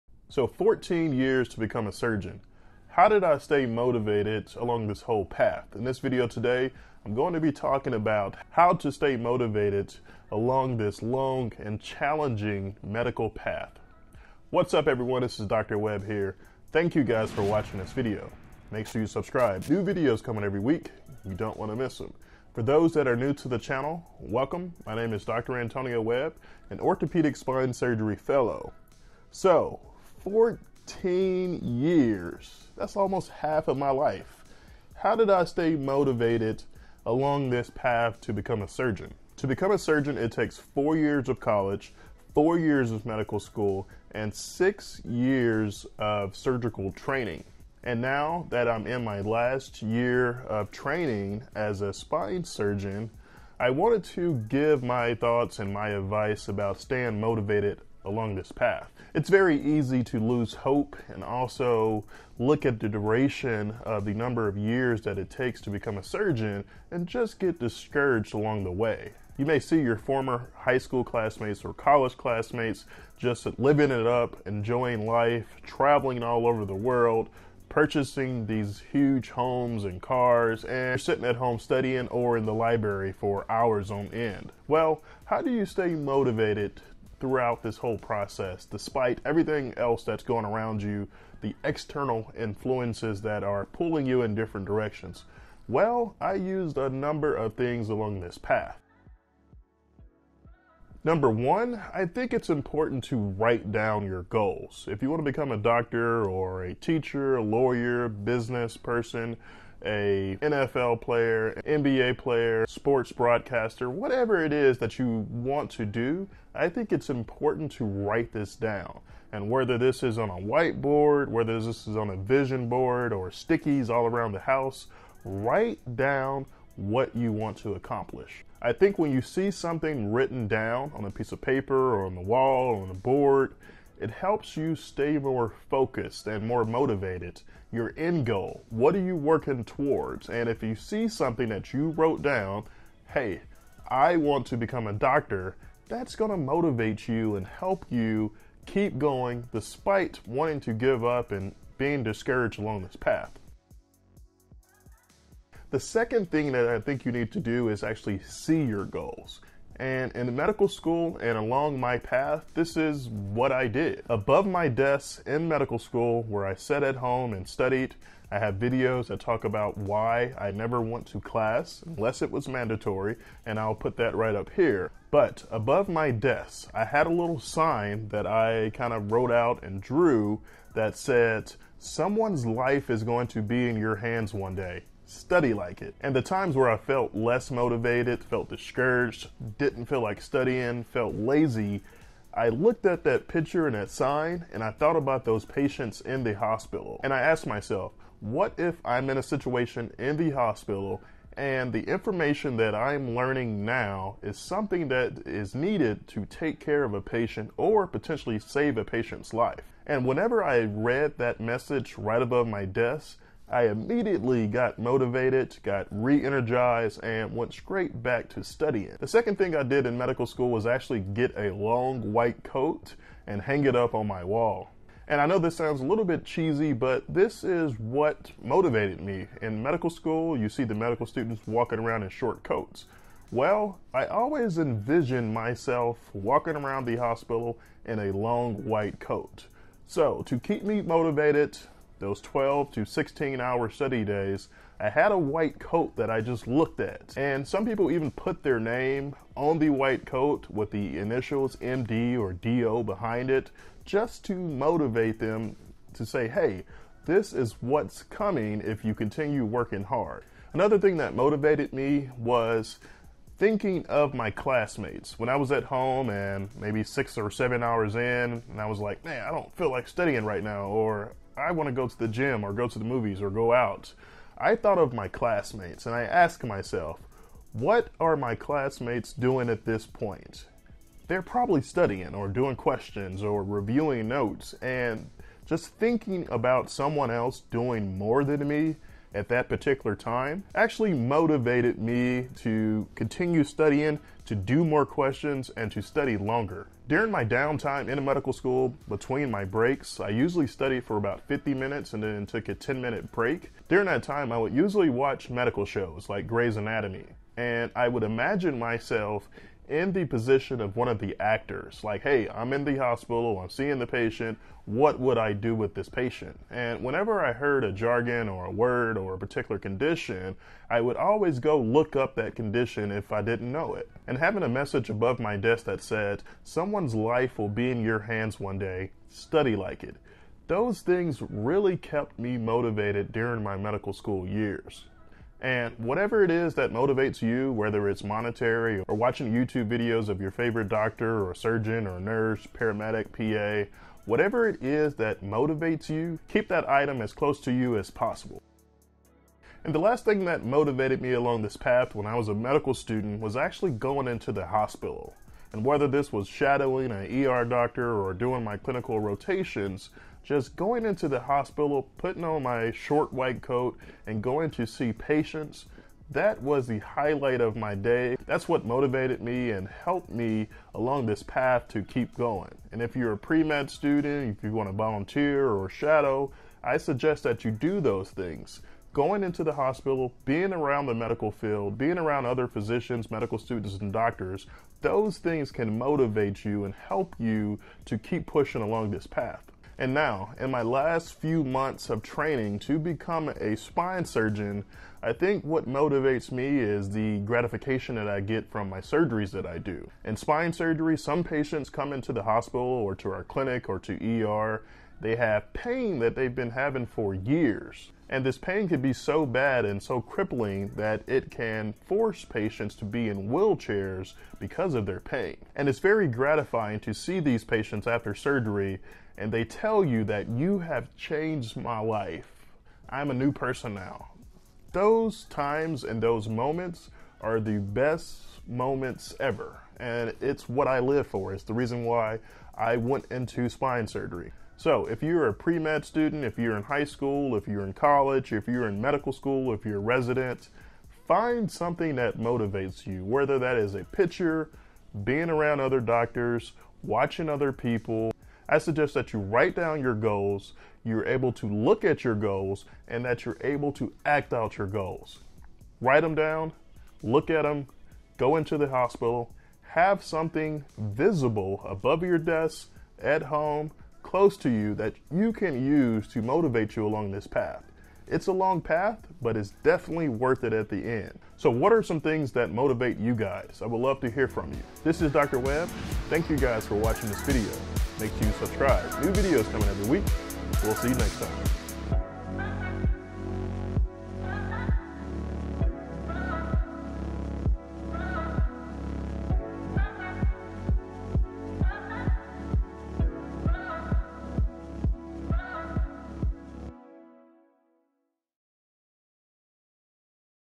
14 YEARS to Become a Surgeon | How I Stay Motivated....
In this video, Dr. Webb talks about the challenging last 14 years of his life to become a surgeon and how he stayed motivated..

4 years of college
4 years of medical school
3-7 years of residency
+/- 1-2 years of fellowship (optional)

Items I Used to Work Out & Stay Healthy in Med School
1/2-Inch Extra Thick Exercise Mat with Carrying Strap

What I Took To Stay Healthy While in Medical School!